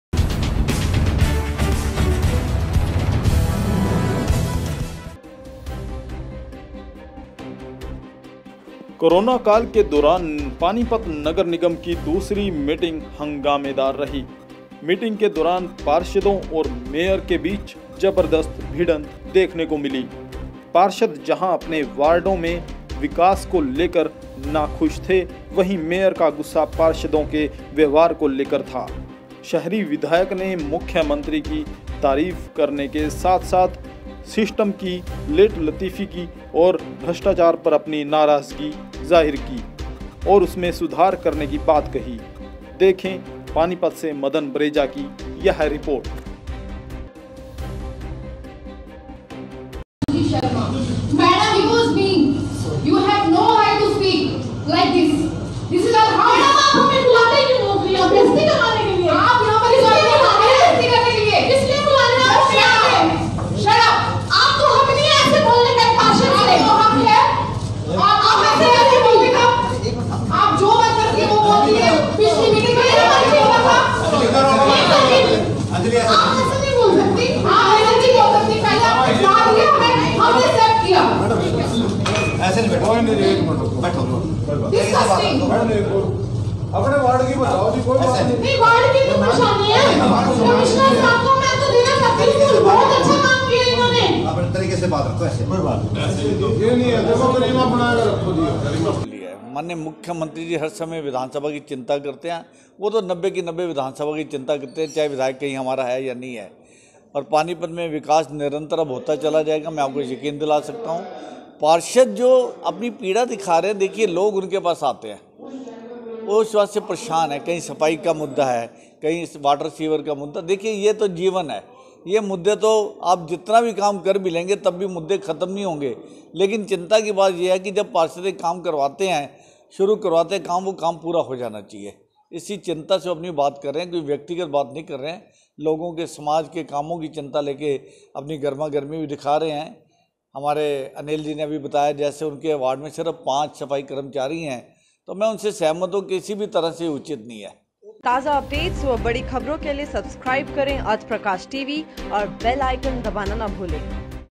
पानीपत निगम की बैठक में हाई वोल्टेज ड्रामा, महिला पार्षद और मेयर आपस में भिड़ी
कोरोना काल के दौरान नगर निगम की दूसरी मीटिंग हंगामेदार रही मीटिंग के दौरान पार्षदों और मेयर के बीच जबरदस्त भिड़ंत देखने को मिली पार्षद जहां अपने वार्डो में विकास को लेकर नाखुश थे वहीँ मेयर का गुस्सा पार्षदों के व्यवहार को लेकर था । शहरी विधायक ने मुख्यमंत्री की तारीफ करने के साथ साथ सिस्टम की लेटलतीफी और भ्रष्टाचार पर अपनी नाराजगी ज़ाहिर की और उसमें सुधार करने की बात कही। देखें पानीपत से मदन बरेजा की यह रिपोर्ट